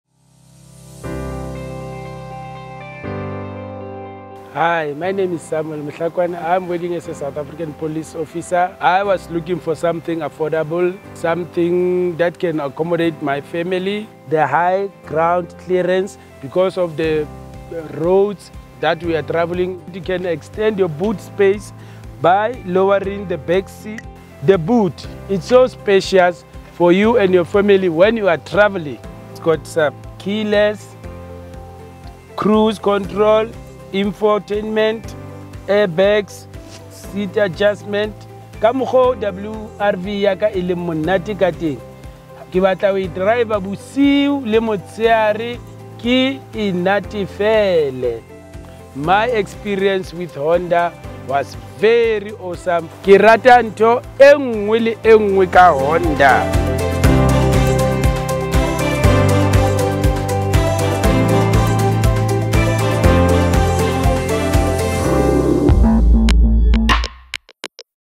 Honda WR-V Testimonial from Jokosele Samuel Mohlakoana
Police officer Jokosele Samuel Mohlakoana, loves his WR-V thanks to all its amazing features like high clearance, push button start and the amazing Honda Magic Seats®.